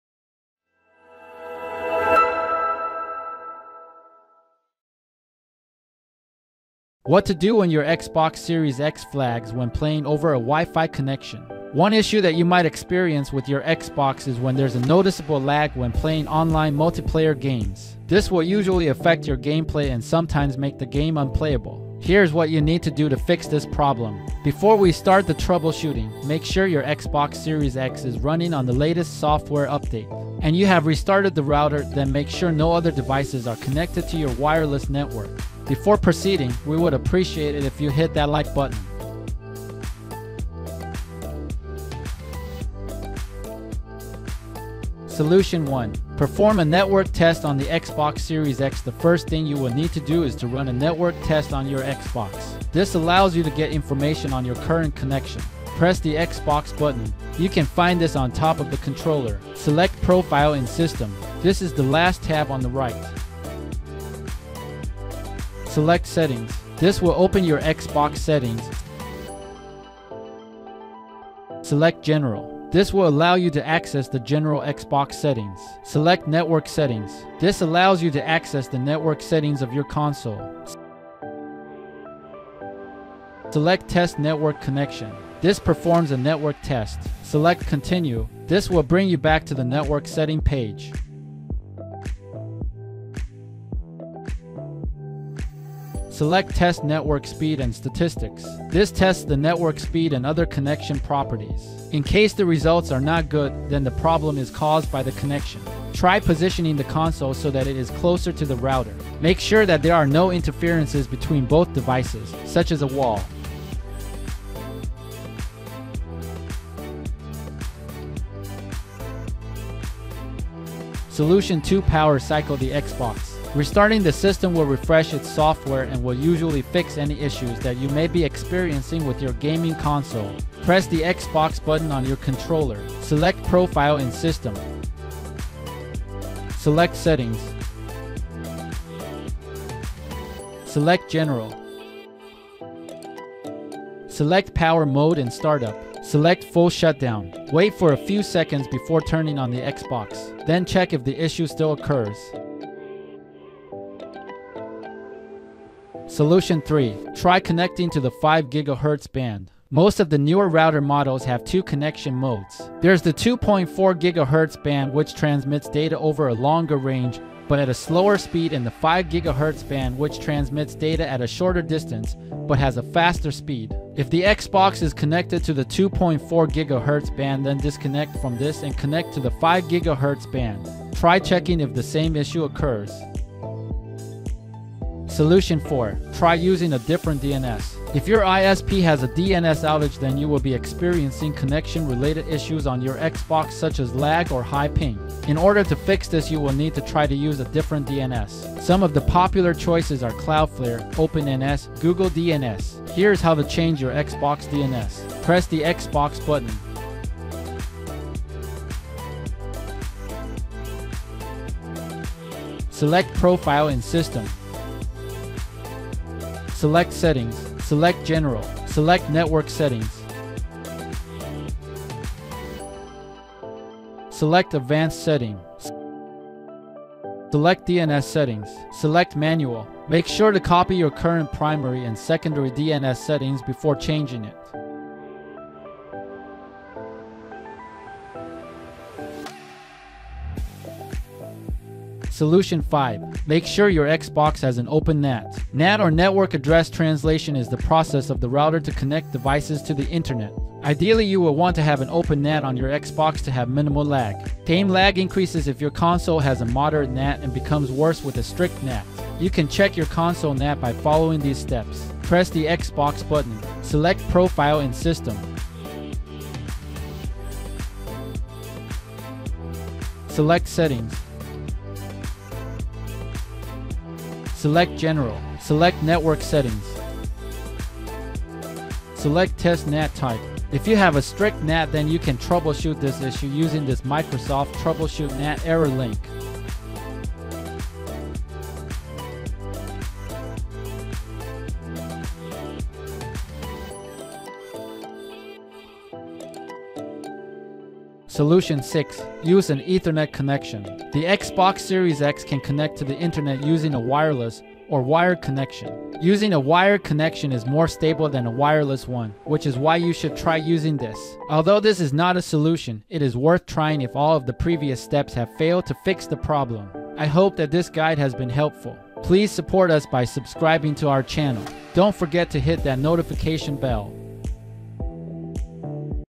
How To Fix Xbox Series X Wi-Fi Lag Problem
On this video, we will show you How To Fix Xbox Series X Wi-Fi Lag Problem

00:45 Perform a network test
02:01 Power cycle the Xbox
02:36 Try connecting to the 5GHz band
03:05 try using a different DNS
04:02 Make sure your Xbox has an open NAT
04:58 Use an Ethernet connection